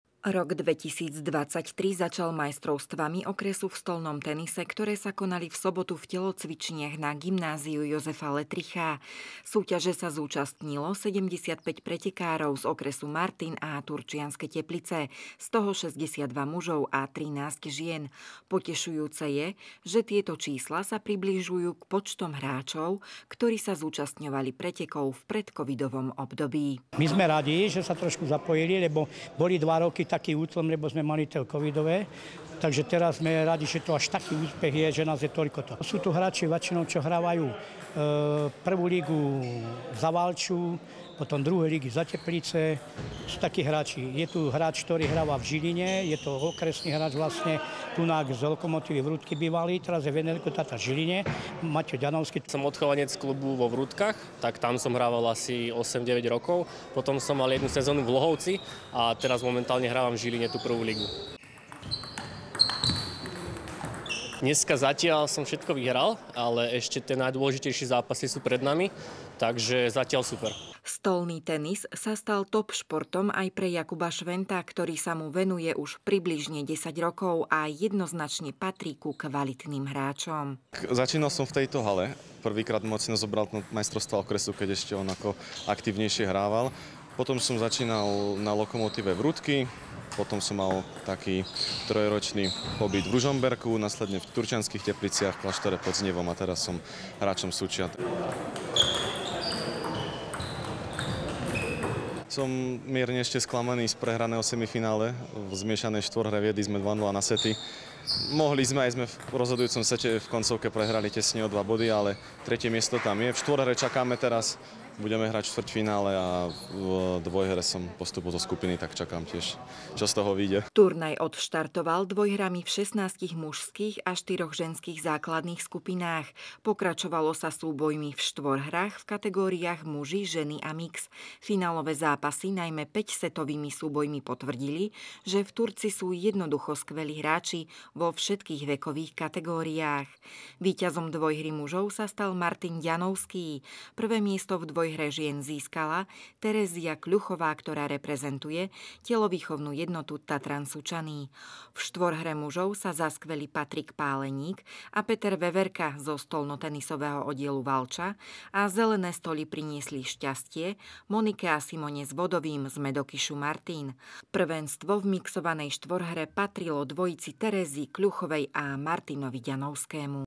Majstrovstvá okresu – regiónu Turiec v stolnom tenise
V sobotu 7. januára 2022 sa konali v telocvičniach na Gymnáziu J. Lettricha v Martine Majstrovstvá okresu – regiónu Turiec v stolnom tenise. Zúčastnilo sa ich 75 pretekárov z toho 62 mužov a 13 žien. O kvalite turnaja nebolo pochýb. Nasvedčovali tomu dramatické finálové zápasy, najmä mužskej dvojhry a štvorhry i ženskej štvorhry, ktoré sa hrali na päť setov.